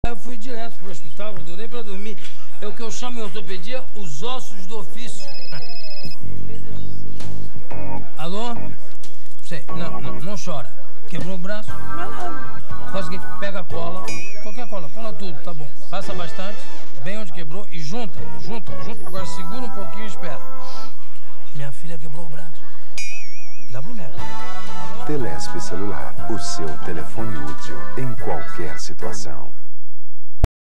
Telesp Celular com Jô Soares - HD Propaganda - Anos 90
Propaganda feita pela Espiral Produtora de George Jonas